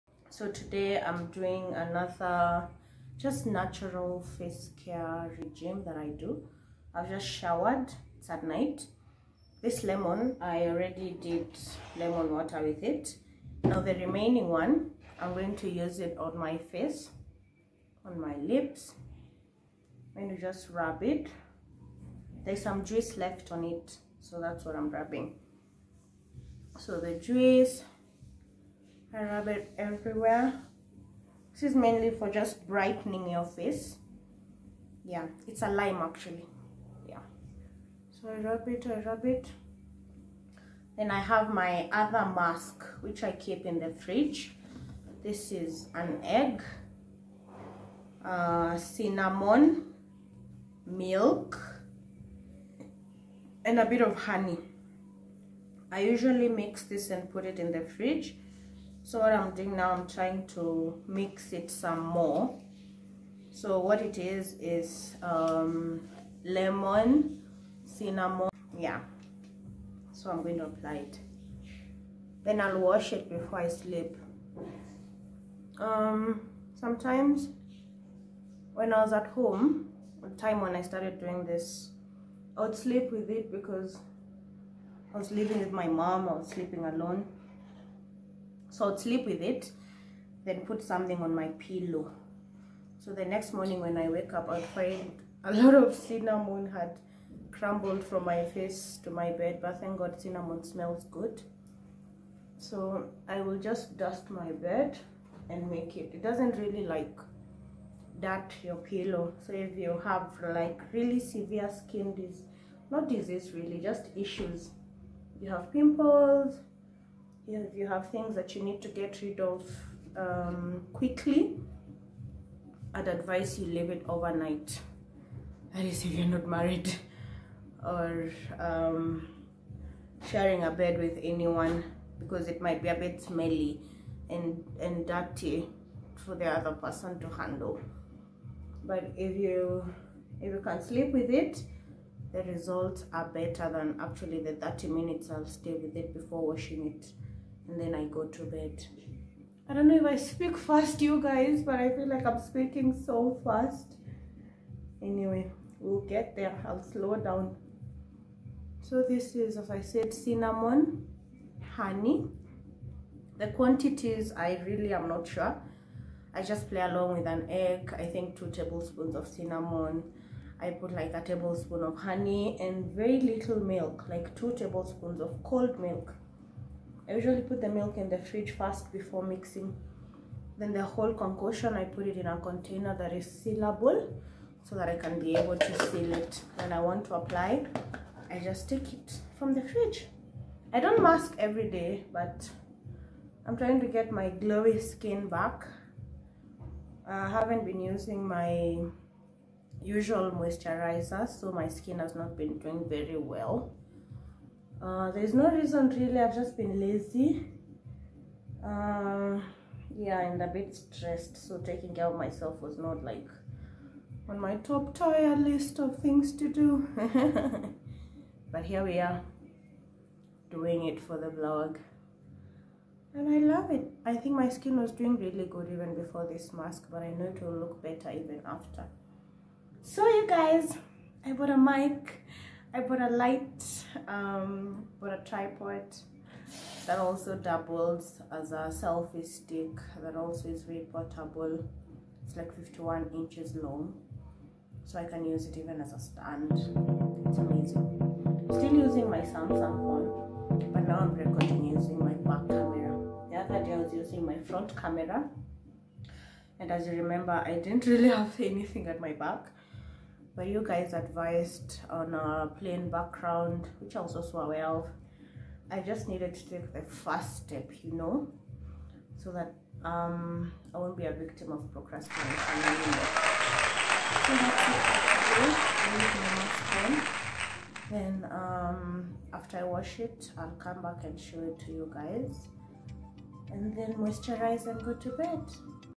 Homemade Cinnamon brightening Mask & Got new Vlog accessories
In this episode. I show you guys how I use cinnamon for a brighter skin. You might not believe this but cinnamon has skin lightening properties. It works towards fading away acne scars, blemishes, marks and dark spots. Cinnamon brings blood to skin surface: This property of cinnamon can help in getting rid of fine lines on the face.